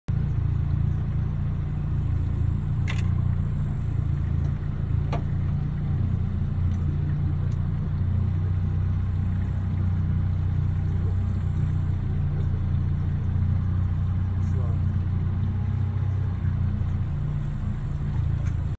Fishing Guide Mike Rasmussen Gives American Shad Demo
Mike Rasmussen, local fishing guide, puts on a demonstration on American Fishing On the Sacramento River near Red Bluff, Ca on June 22, 2019.